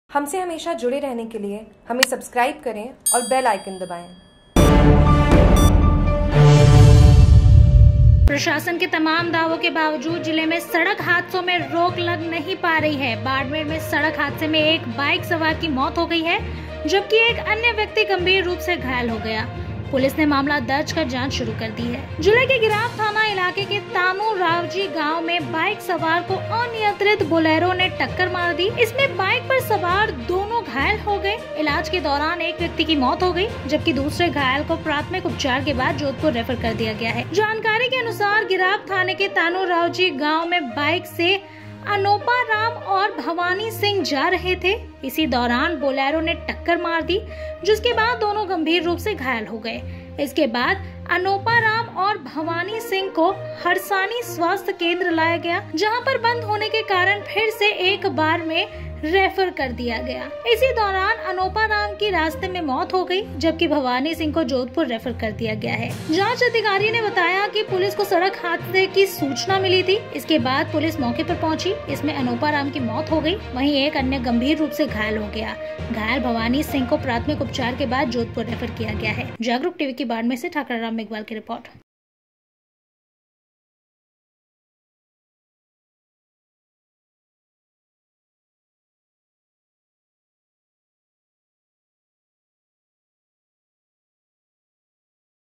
बाड़मेर में सड़क हादसे में बाइक सवार की मौत
प्रशासन के तमाम दावों के बावजूद जिले में सड़क हादसों में रोक लग नहीं पा रही है. बाड़मेर में सड़क हादसे में एक बाइक सवार की मौत हो गई है. जबकि एक अन्य व्यक्ति गंभीर रूप से घायल हो गया. पुलिस ने मामला दर्ज कर जांच शुरू कर दी है.

जिले के गिराब थाना इलाके के तानु रावजी गांव में बाइक सवार को अनियंत्रित बोलेरो ने टक्कर मार दी. इसमें बाइक सवार पर सवार दोनों घायल हो गए. इलाज के दौरान एक व्यक्ति की मौत हो गई. जबकि दूसरे घायल को प्राथमिक उपचार के बाद जोधपुर रेफर कर दिया गया है.

जानकारी के अनुसार गिराब थाने के तानु राव जी गांव में बाइक से अनोपाराम और भवानी सिंह जा रहे थे. इसी दौरान बोलेरो ने टक्कर मार दी जिसके बाद दोनों गंभीर रूप से घायल हो गए.

इसके बाद अनोपाराम और भवानी सिंह को हरसाणी स्वास्थ्य केंद्र लाया गया जहां पर बंद होने के कारण फिर से एक बार में रेफर कर दिया गया. इसी दौरान अनोपाराम की रास्ते में मौत हो गई, जबकि भवानी सिंह को जोधपुर रेफर कर दिया गया है.जांच अधिकारी ने बताया कि ने कि पुलिस को सड़क हादसे से की सूचना मिली थी. इसके बाद पुलिस मौके पर पहुंची. इसमें अनोपाराम की मौत हो गई. वहीं एक अन्य गंभीर रूप से घायल हो गया. घायल भवानी सिंह को प्राथमिक उपचार के बाद जोधपुर रेफर किया गया है.